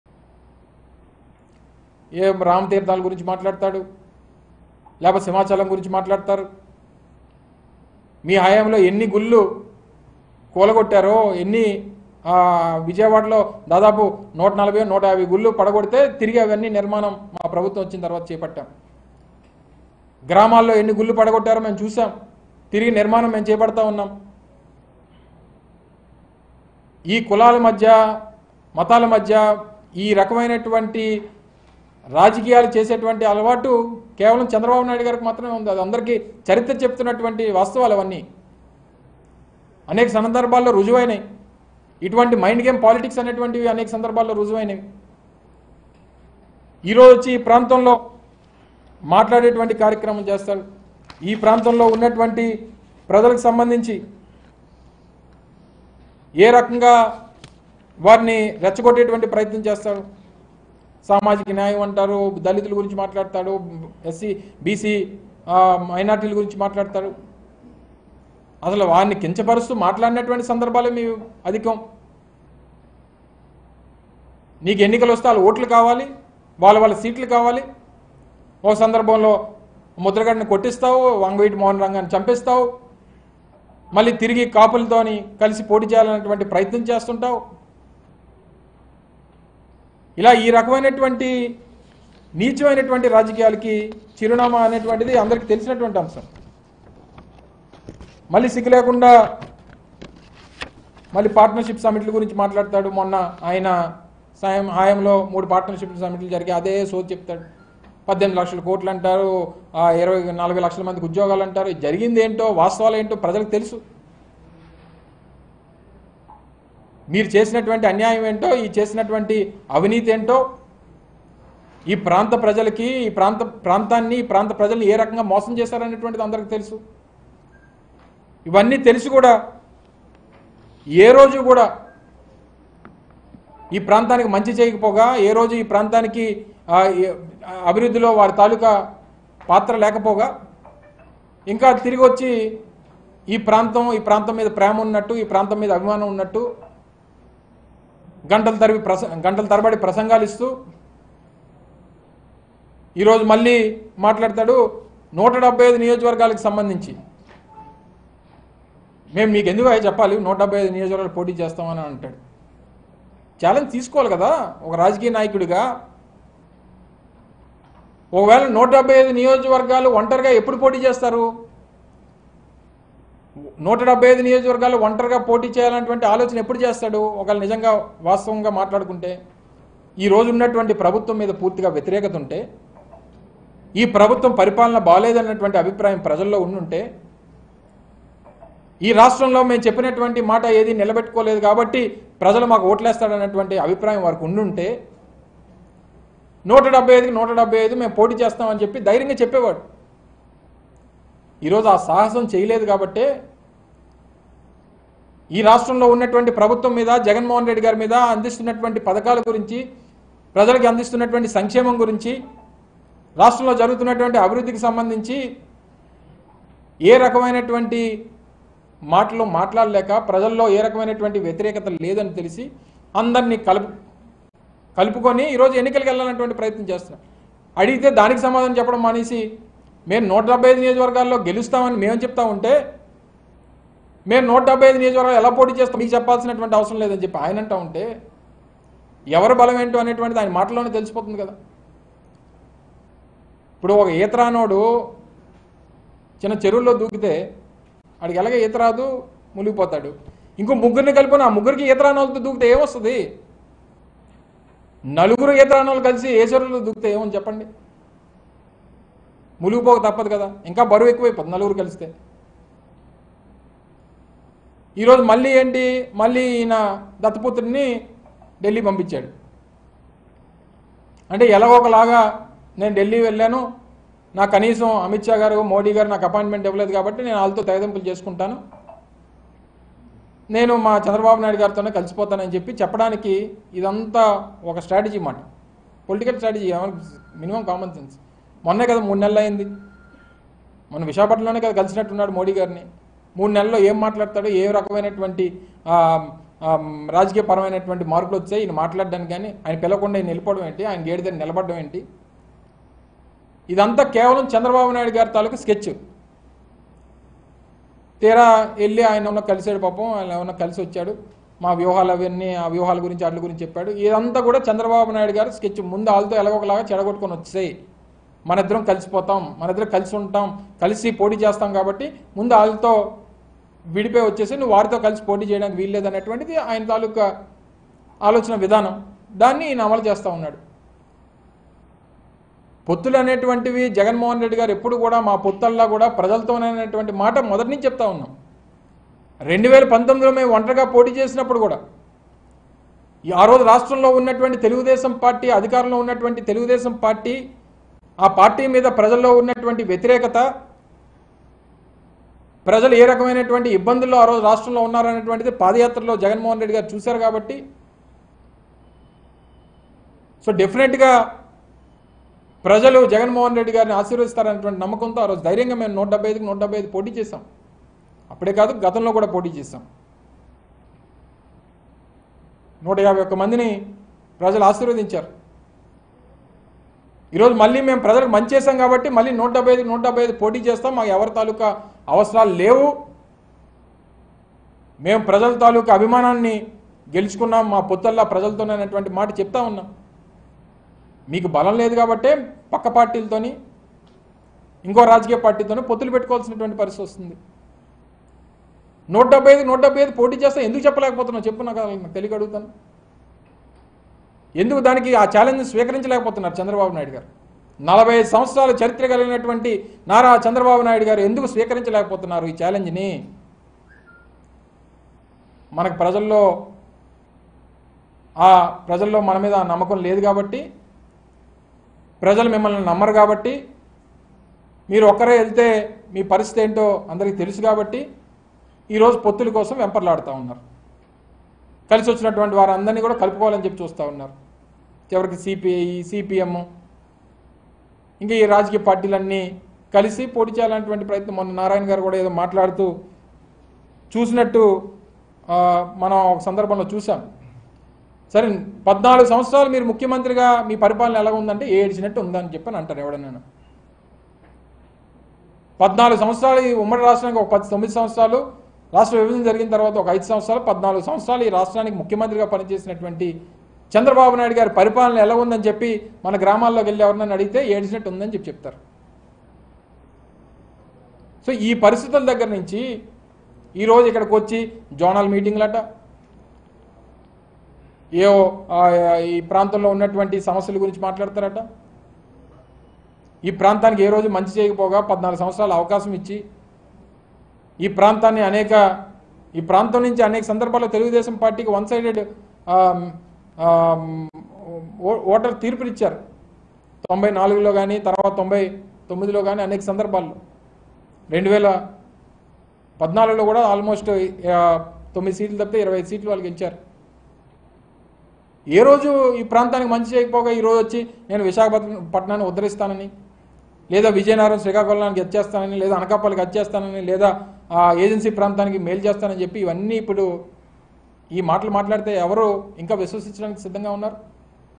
Minister Gudivada Amarnath Comments On Pawan Kalyan & Chandrababu Over Alliances | Mango News
Minister Gudivada Amarnath Comments On Pawan Kalyan & Chandrababu Over Alliances.